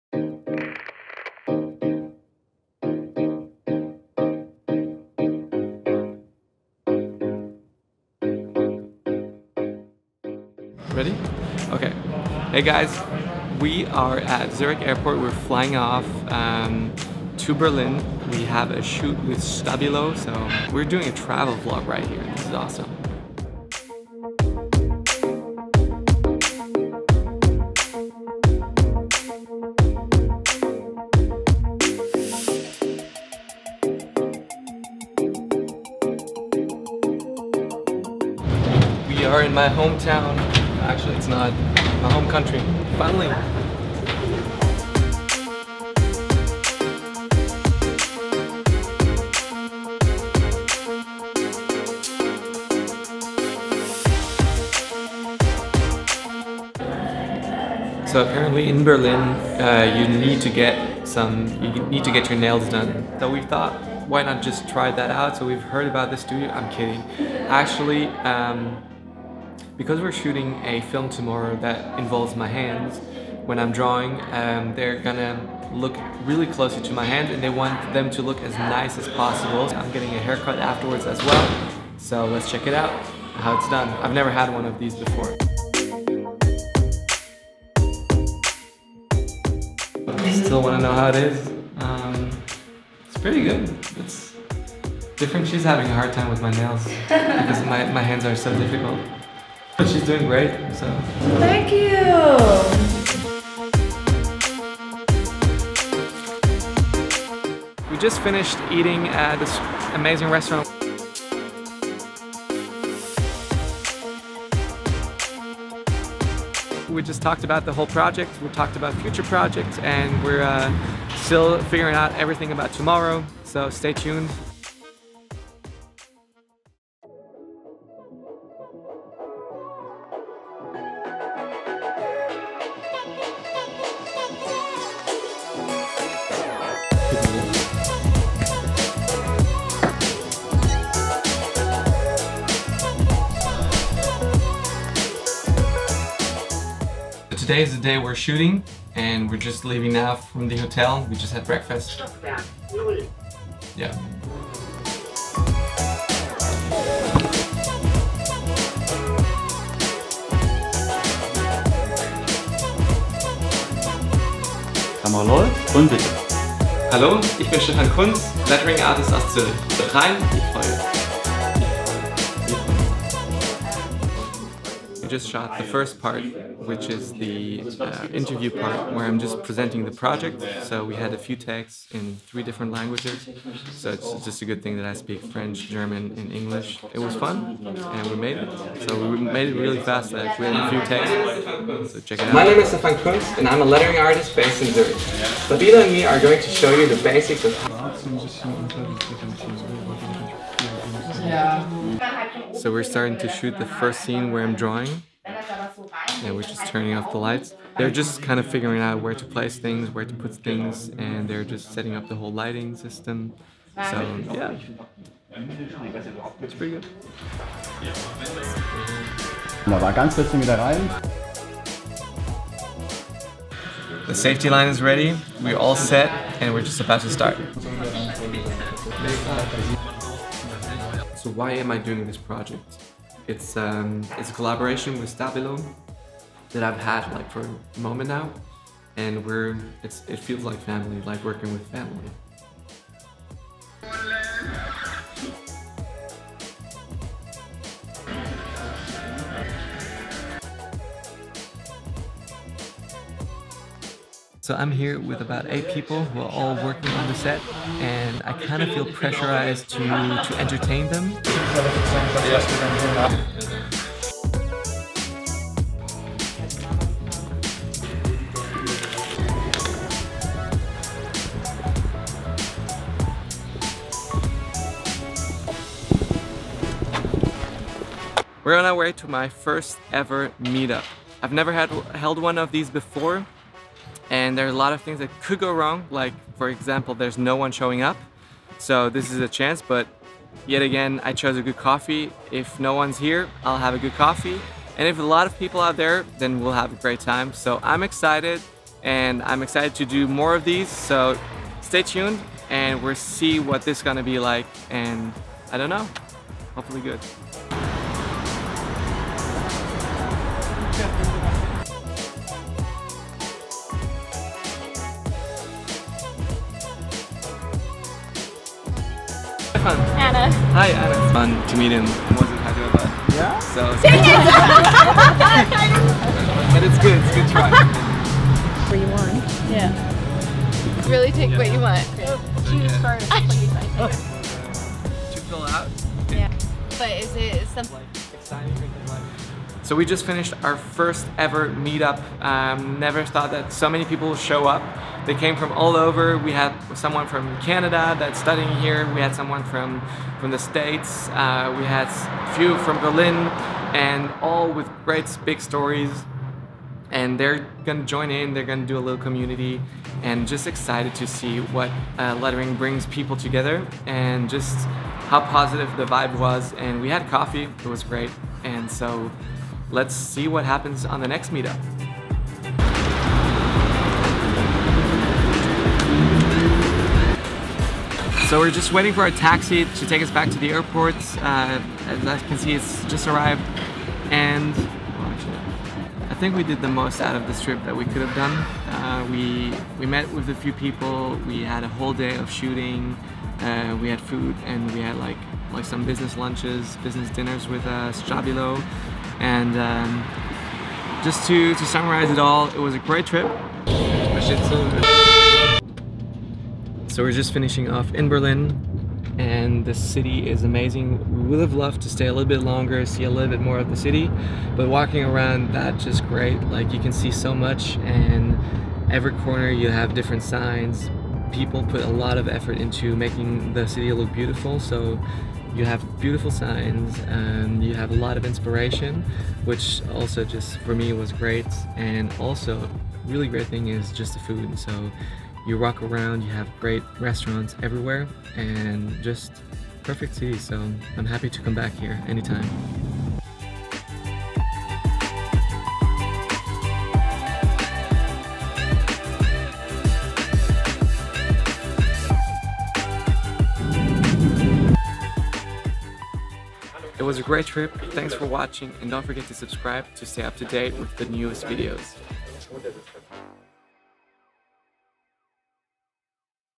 8 HOURS OF DRAWING IN BERLIN, Germany
I've been working for over a month on this project with Stabilo, only to get a manicure and haircut in Berlin, shooting for a whole day. IT WAS AMAZING!

all vom joakim karud